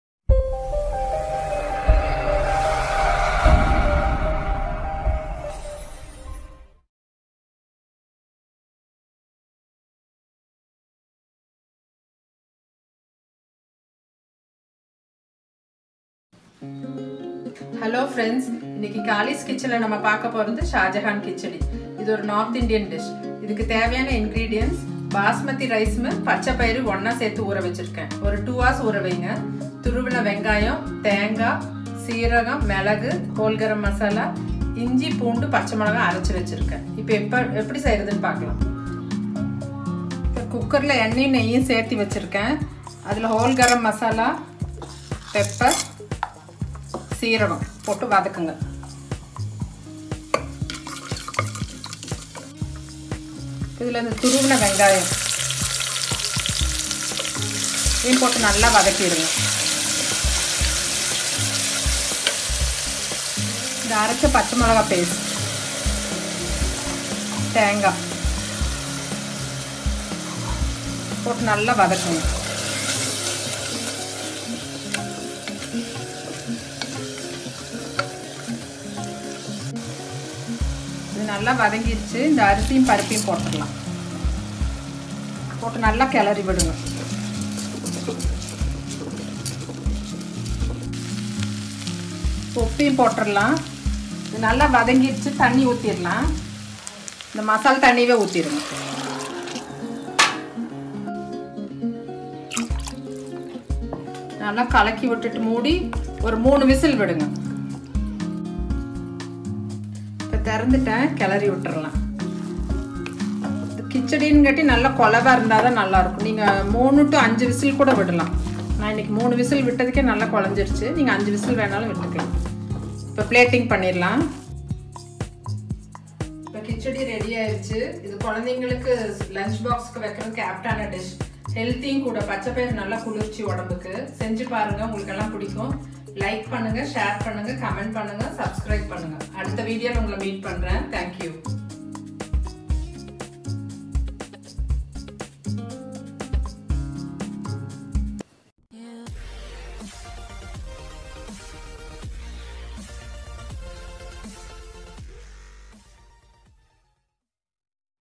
Shahjahan Kichedi Receipe in Tamil | Dhal pulav Receipe | Pulav Receipes in Tamil
Today’s special - Shahjahan Kichedi Receipe best suits with non veg gravy and raitha. Easy to do within few minutes. Impress your friends and guests with this receipe.

Ingredients required:

1. Basmati rice - 1/2 cup

2. Green gram dhal - 1/2 cup

3. Cinnamon, cardamom and clove - 2 each

4. Ginger garlic green chilli paste - 2 tbs

5. Cumin - 1 tbs

6. Pepper - 1/2 tbs

7. Grated coconut - 1/4 cup

8. Grated onion - 1/4 cup

9. Water - 4 cups

10. Ghee , oil and salt

Procedure:

1. Soak rice and dhal for 2 hours.

2. Heat oil and ghee in a cooker. Add the spices, pepper, onion, ginger garlic green chilli paste, curry leaves, cumin and coconut.

3. Saute well. Now add the rice 4 cups water. Leave 5 whistles. Sprinkle coriander leaves and serve hot.

Happy Cooking!!!